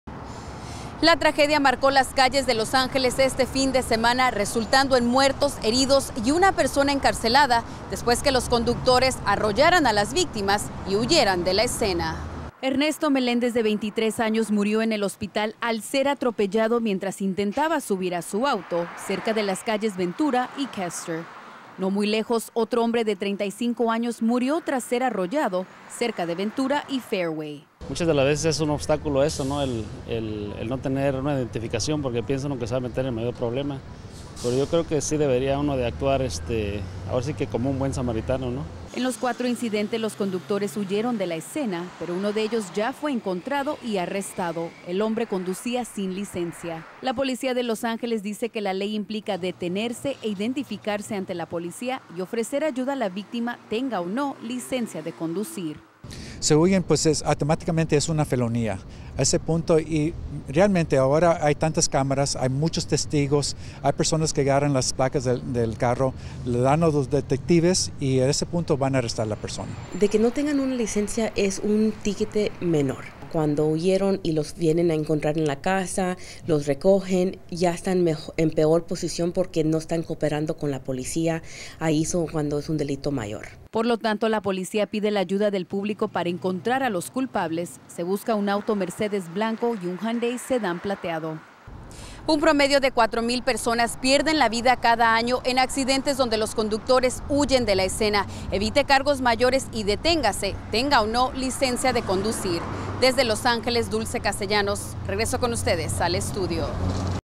Conductores Que Se Dieron a La Fuga
LA POLICIA REALIZA UNA INTENSA BUSQUEDA DE LOS RESPONSABLES
DE ATROPELLOS MORTALES A PEATONES EN EL FIN DE SEMANA